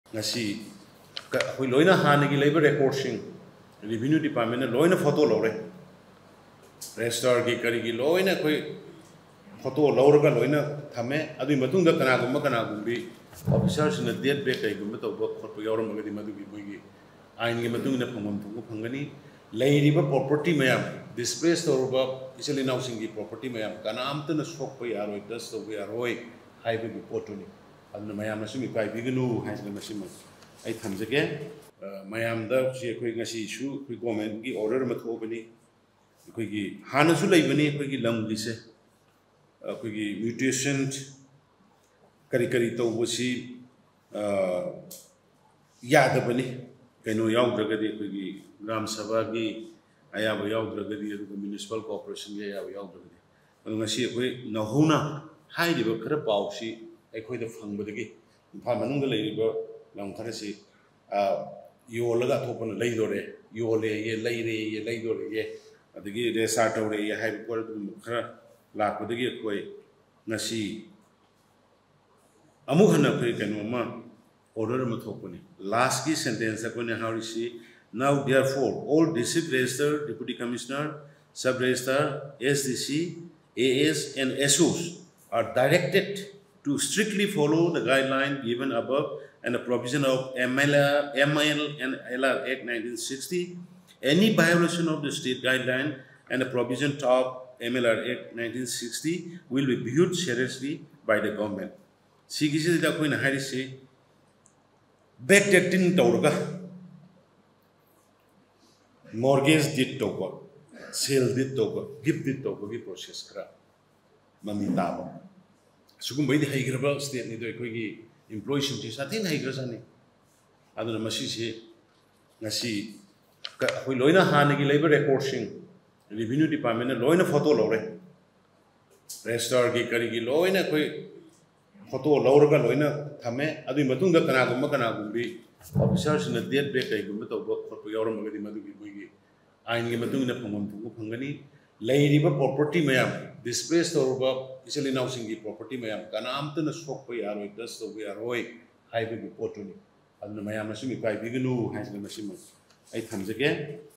Prohibition imposed on buying and selling of land:CM
Prohibition imposed on buying and selling of land and properties whose owners have fled during the unrest: CM